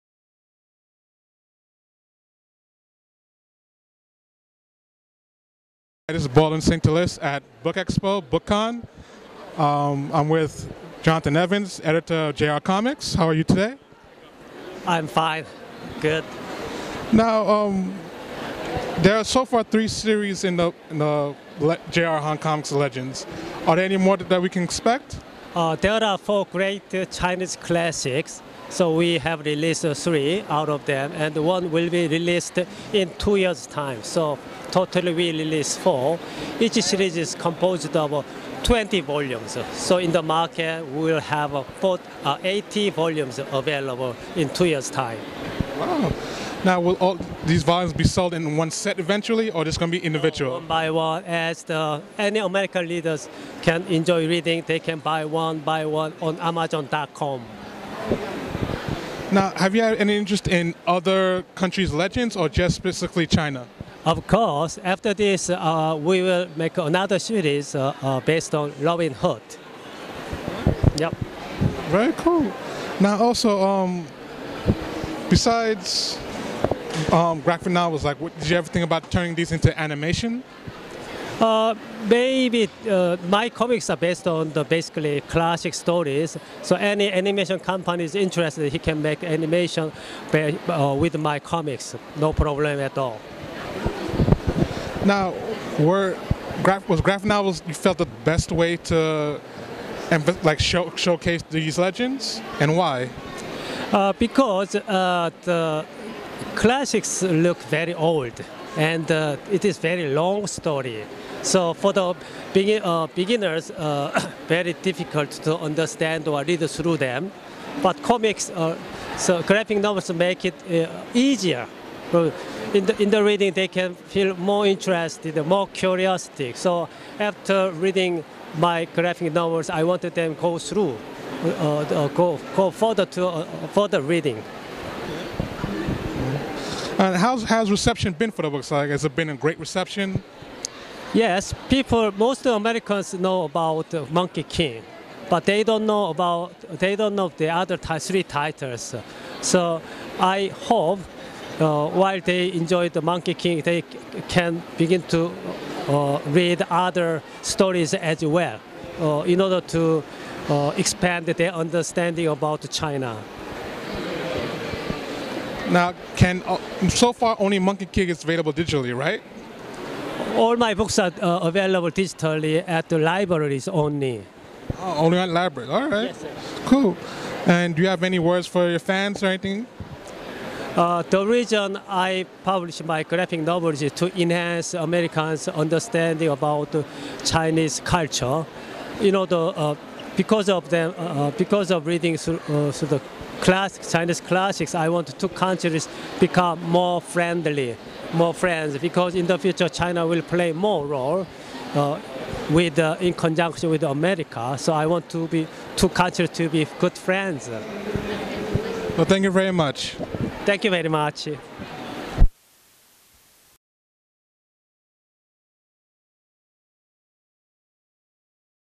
Interview with publisher JR Han
Interview with JR Han on the graphic novels he is publishing based on Chinese legends.

Camera work by Becki O